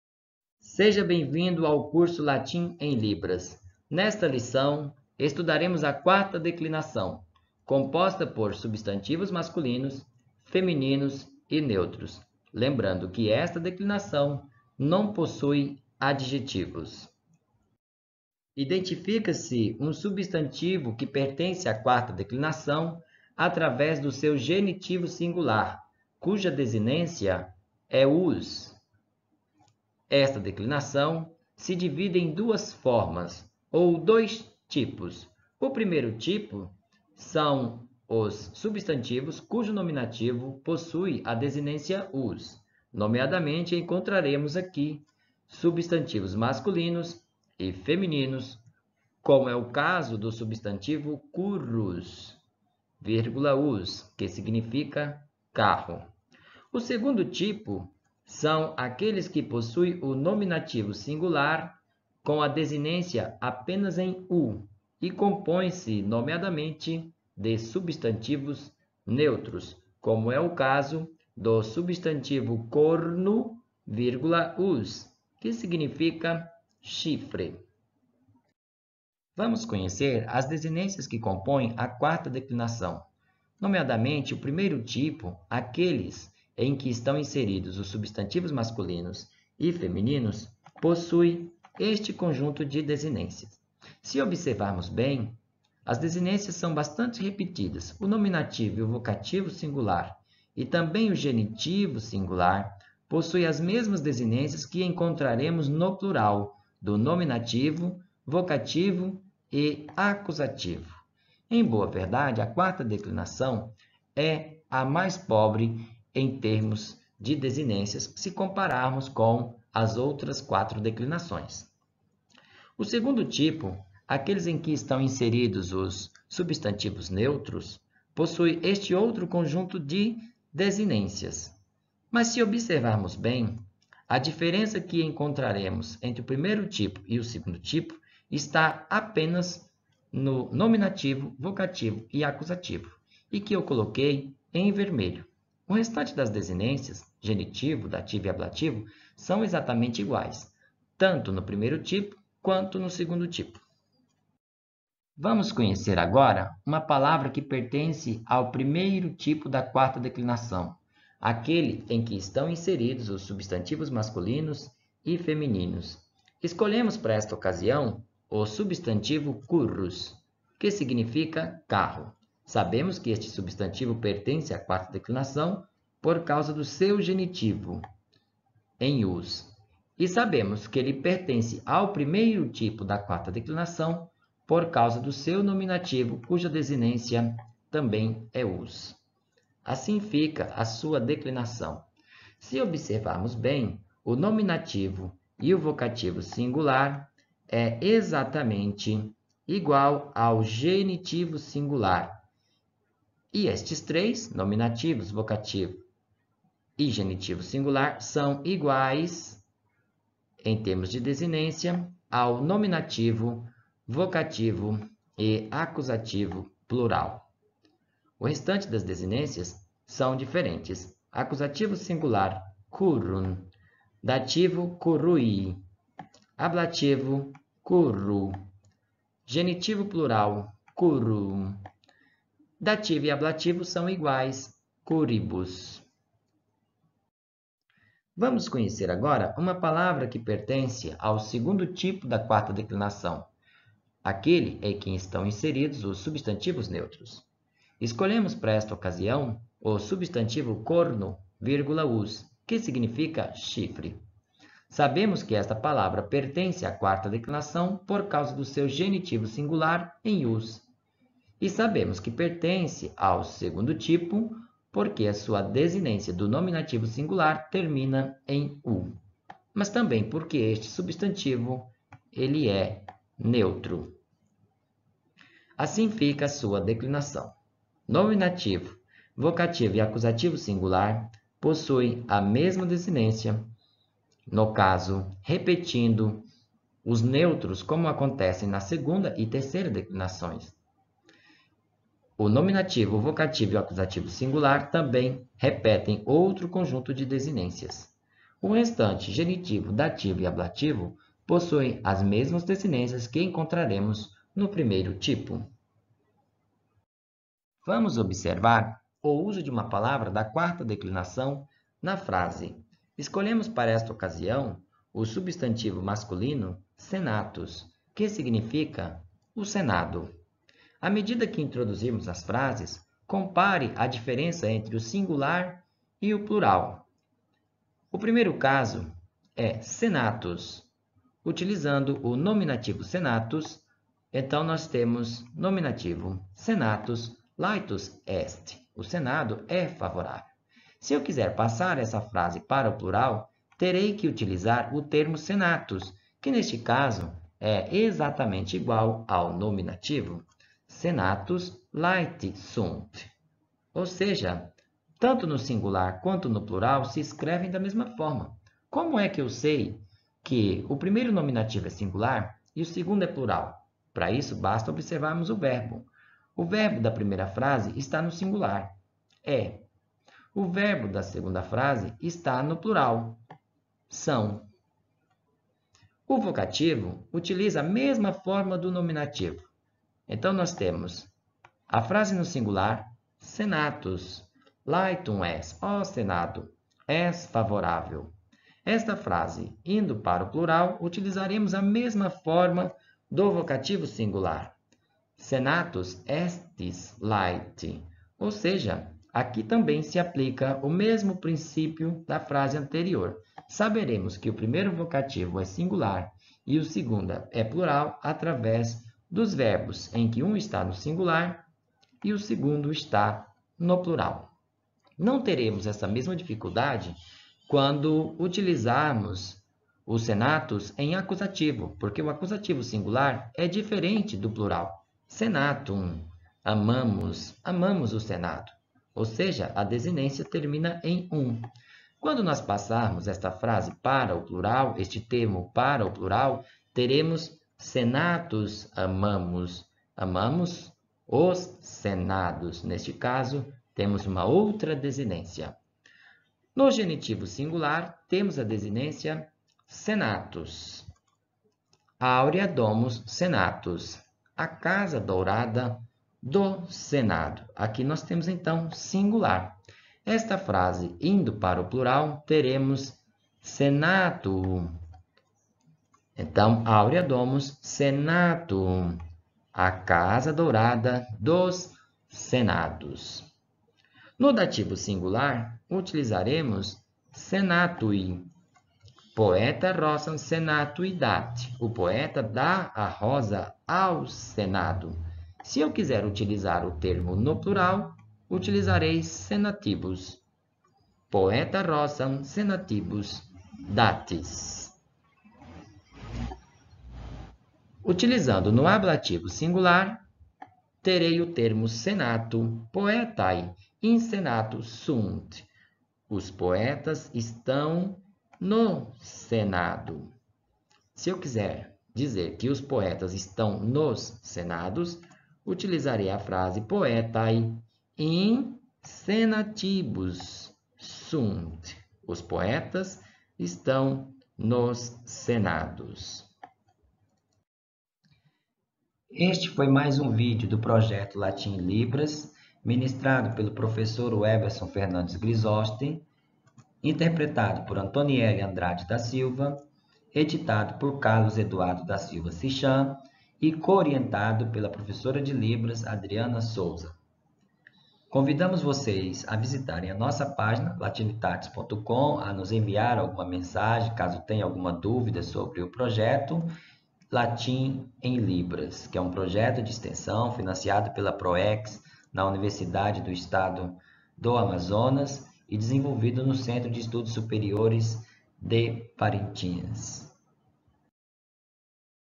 Latim em LIBRAS: Lectio VIII - Quarta Declinação: Substantivos
Projeto de Extensão "Latim em LIBRAS" (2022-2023), orientado pelo Professor Doutor Weberson Fernandes Grizoste, desenvolvido pela acadêmica do curso de Letras, Antoniely Andrade da Silva, do Centro de Estudos Superiores de Parintins, da Universidade do Estado do Amazonas.